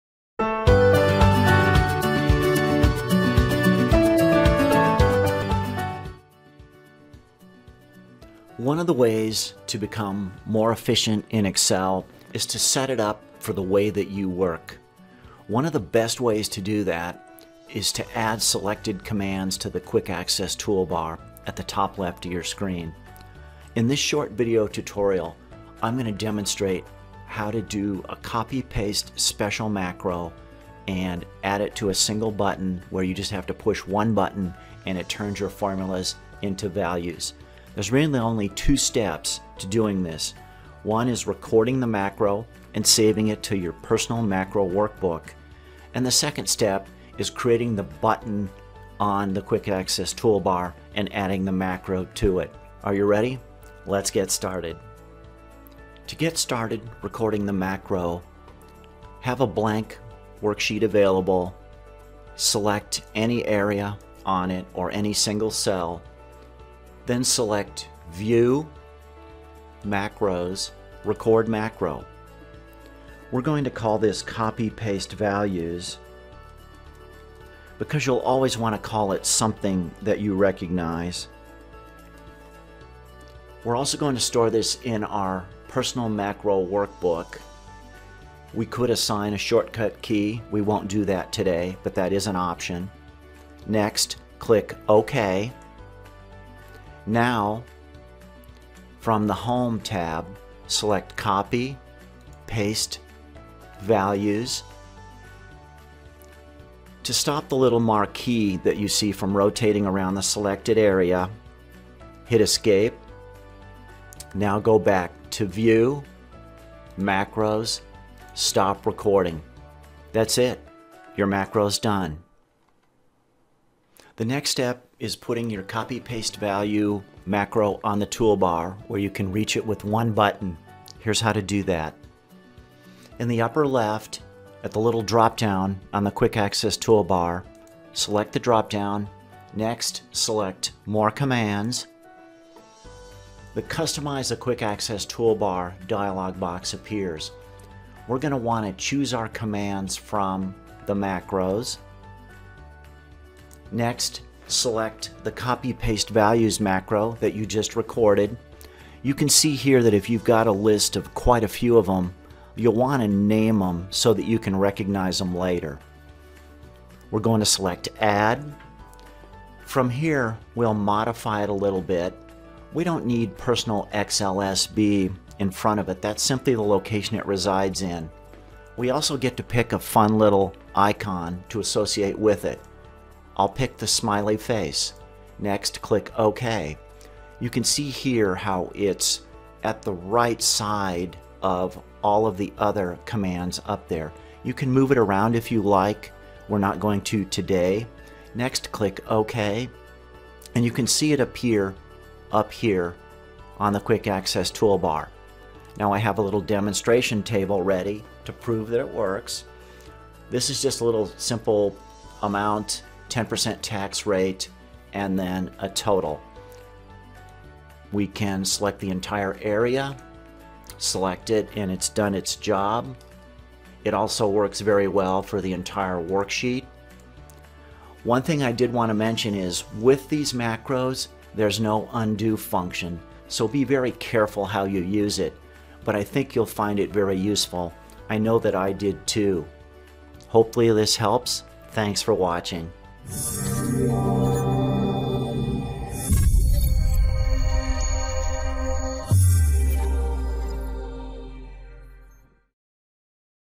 Copy Paste Values With a Single Click
One of the ways to become more efficient in Excel is to minimize the effort and time involved in repetitive tasks. This short tutorial shows how to record a copy paste values macro and put it on the quick access toolbar. Once that's done, just select the range and push the button!